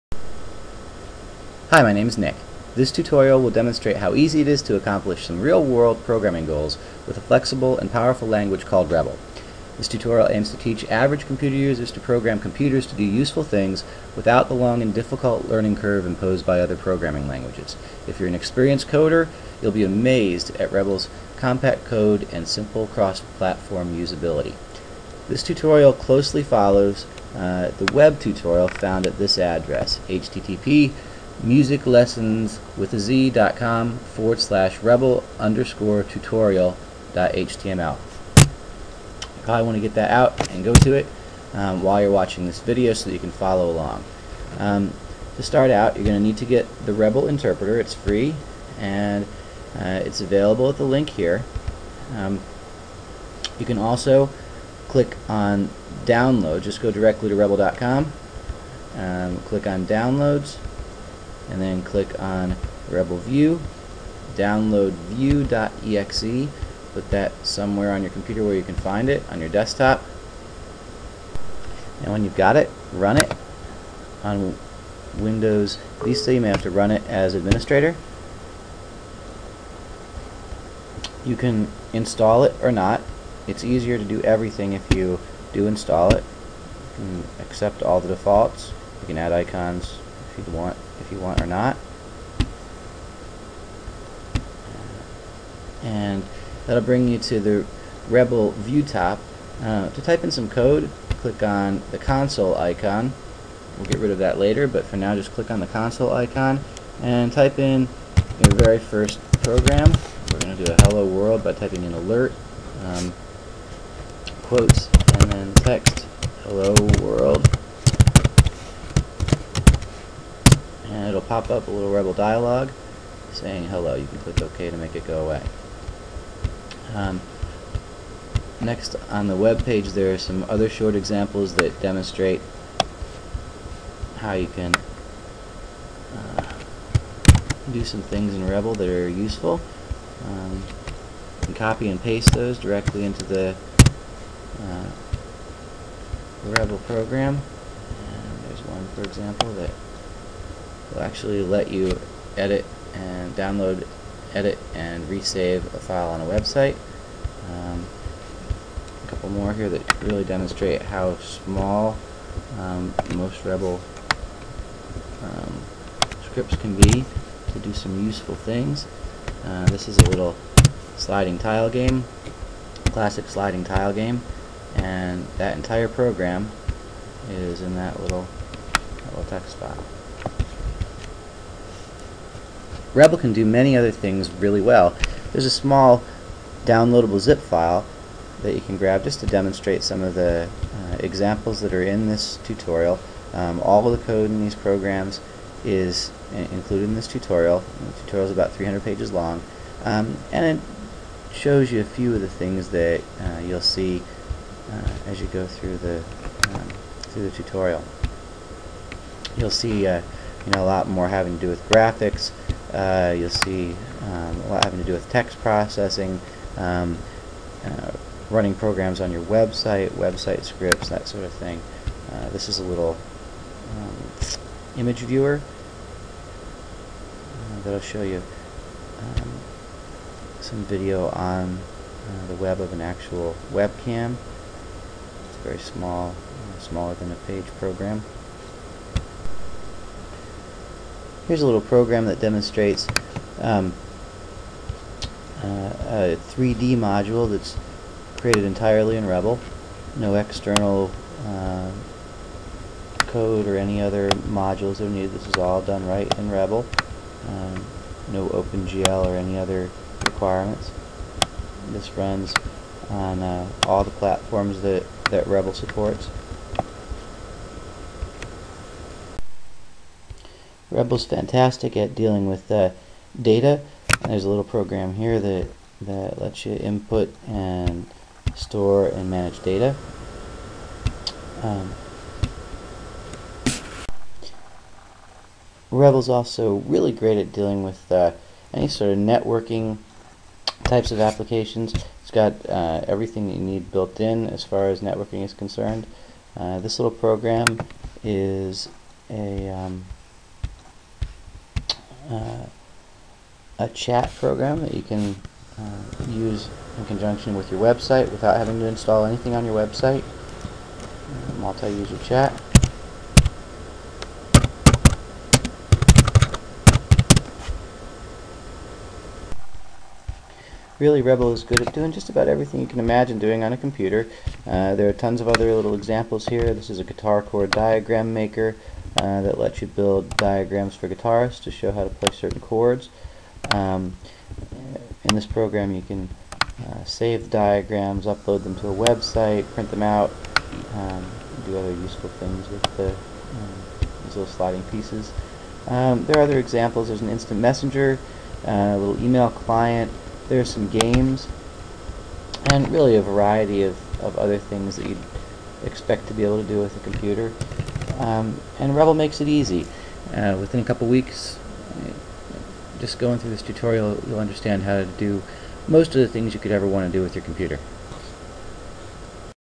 Computer Programming Tutorial, by Nick Antonaccio (#1)
Learn to Create Computer Programs - INTRODUCTION

Here are links to this entire video series (currently more than 8 hours total):

These tutorials demonstrate how easy it is to accomplish real world programming goals with a flexible and powerful language called Rebol.  The video tutorial closely follows the written text at:

which aims to teach average users to program computers to do useful things, without the long and difficult learning curve imposed by other programming languages.  If you're an experienced programmer, it's strongly recommended that you read the more concise tutorial at

You'll be amazed at Rebol's compact code and simple cross-platform usability.